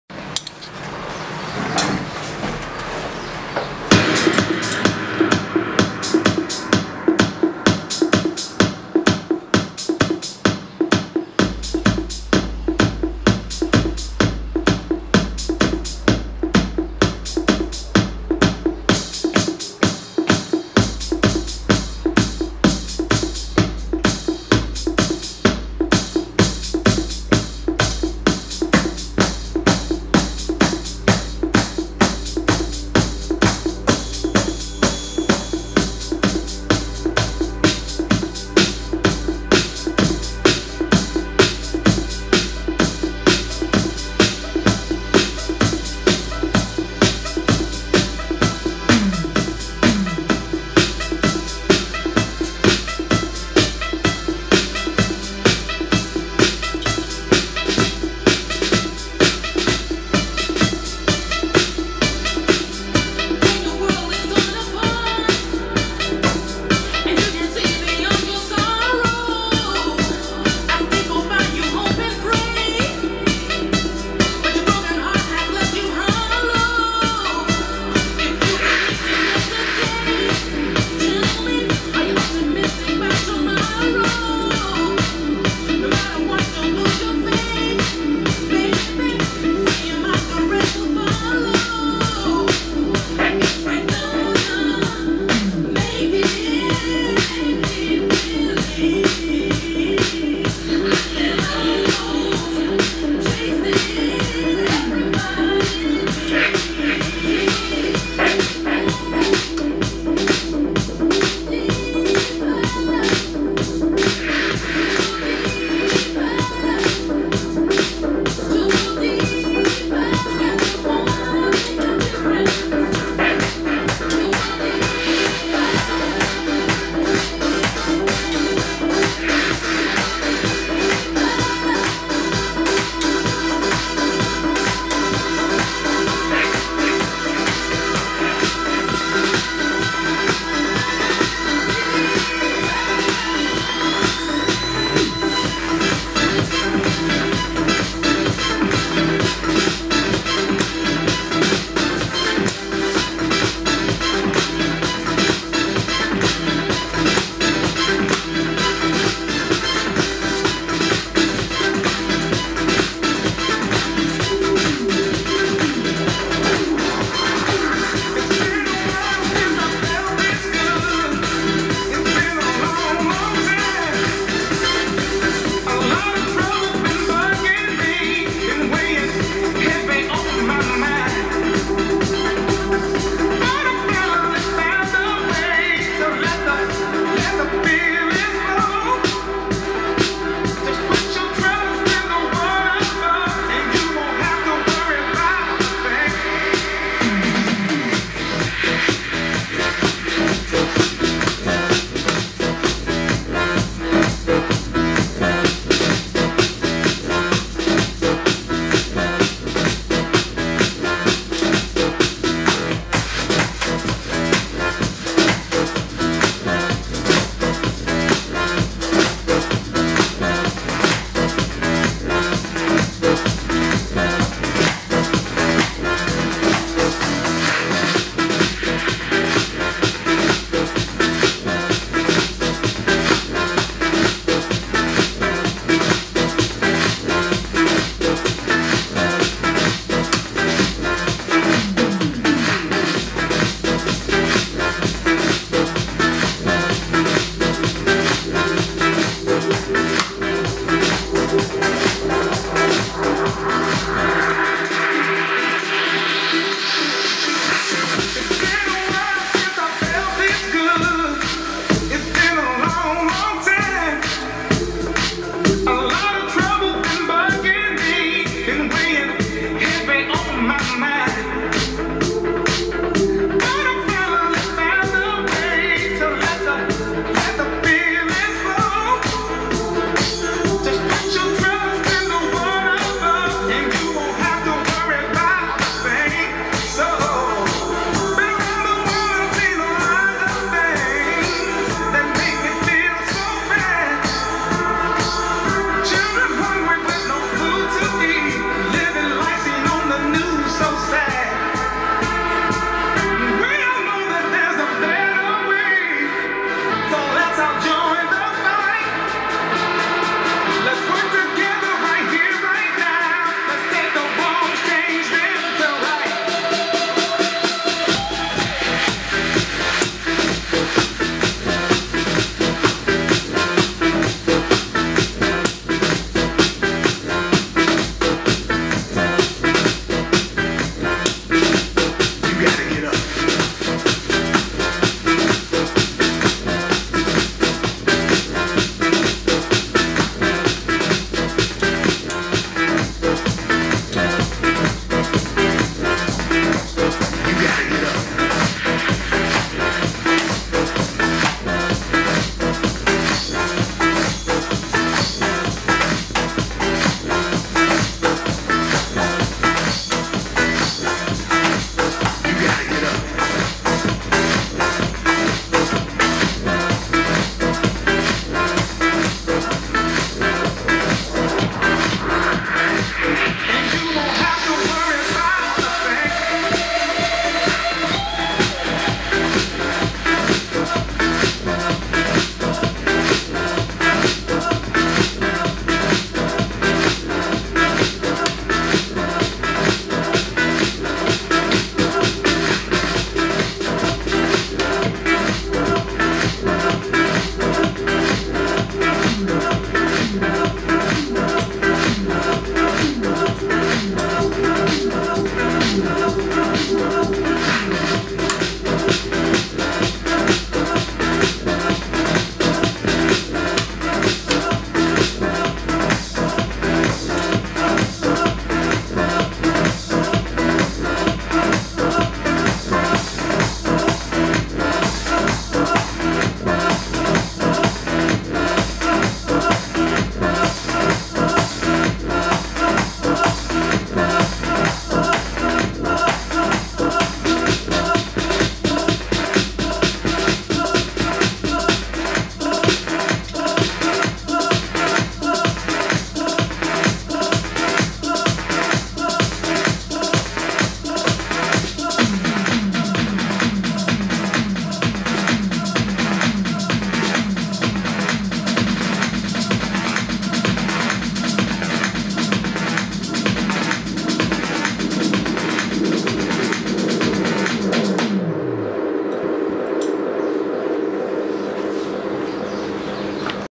DJ Jean Duarte primeiro teste com a Roland HD-1 e suas Colheres...
Este foi apenas um teste, detalhes das colheres de improsivo como baquetas. Pessoal ta ruim.. mas vai ficar bom hehehe